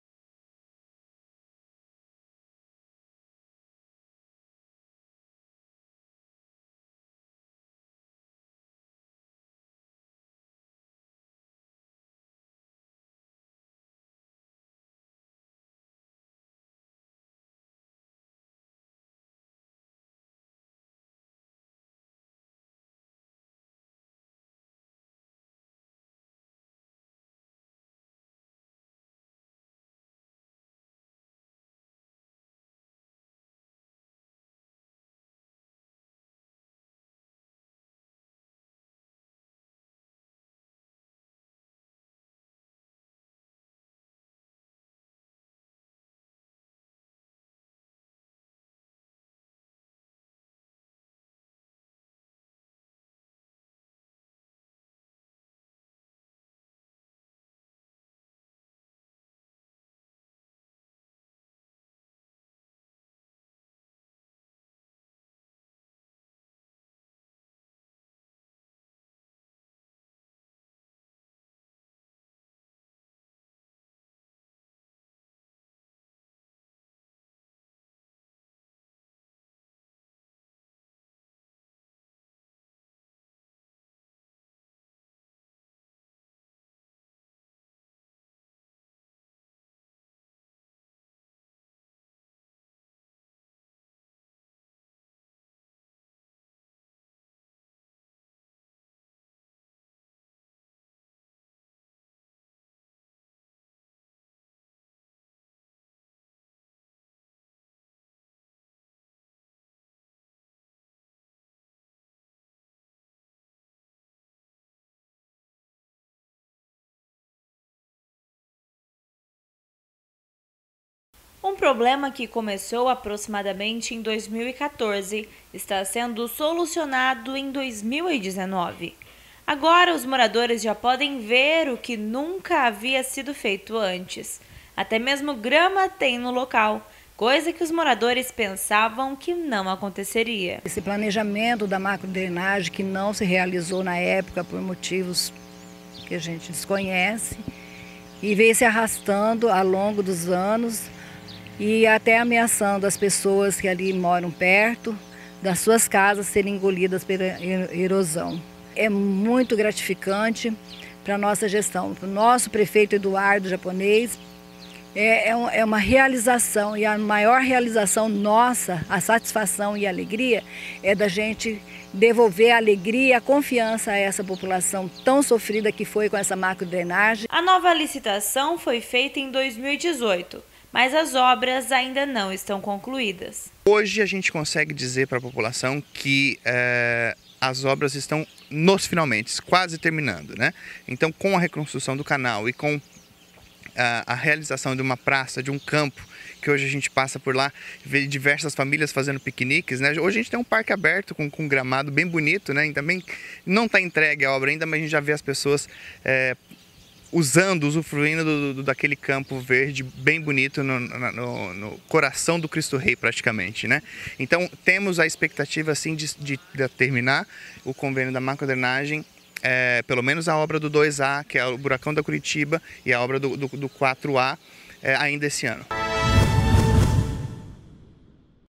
Veja uma retrospectiva do "buraco da avenida Curitiba". Como era? Como está hoje? Confira
Reportagem: Maelly Nunes
Cinegrafia: Joel Jonas
Edição: Everton Mello